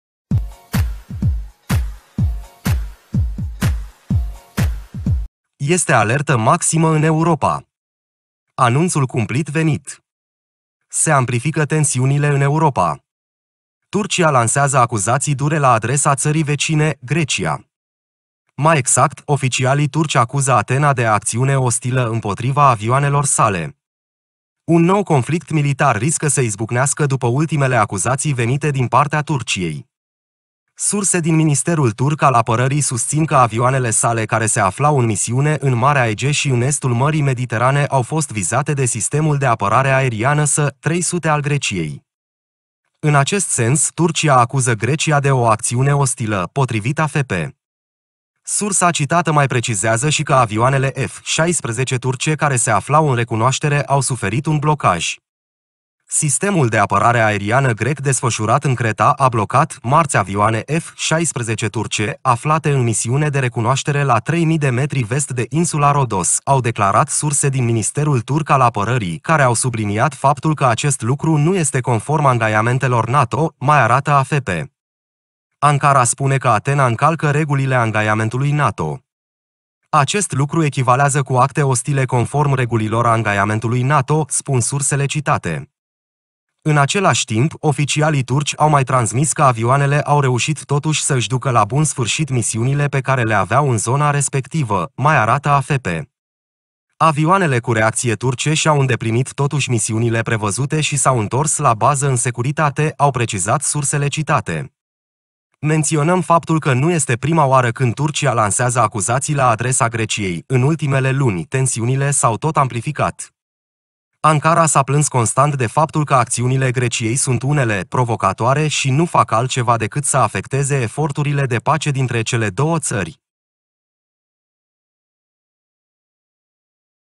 Este alertă maximă în Europa! Anunțul cumplit a venit
Se amplifică tensiunile în Europa. Turcia lansează acuzații dure la adresa țării vecine, Grecia. Mai exact, oficialii turci acuză Atena de ''acţiune ostilă'' împotriva avioanelor sale.